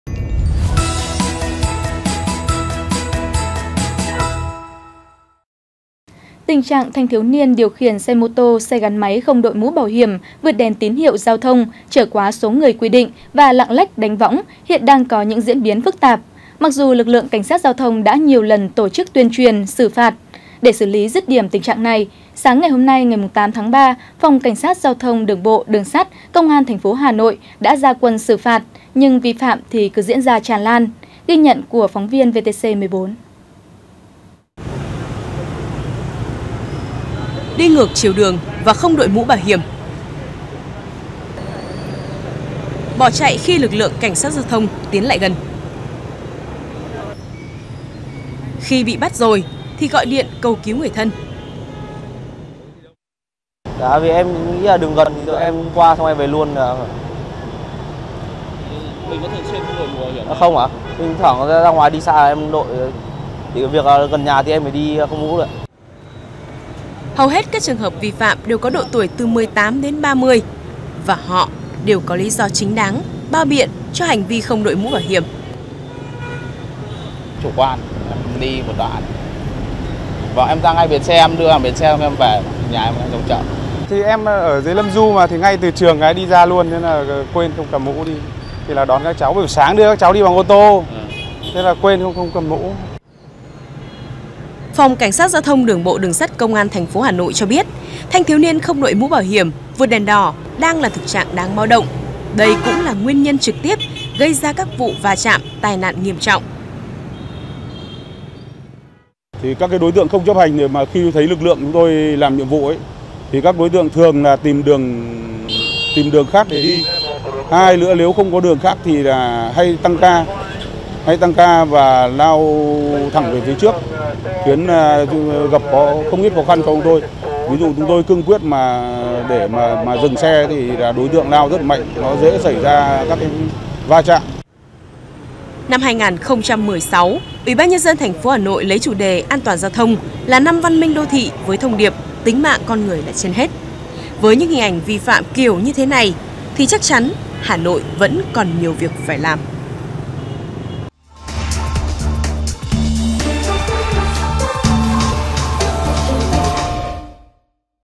Xử lý nghiêm thanh niên không đội mũ bảo hiểm | VTC
VTC | Cảnh sát Giao thông Hà Nội xử lý nghiêm thanh niên có hành vi không đội mũ bảo hiểm, lạng lách, đánh võng khi đi xe gắn máy.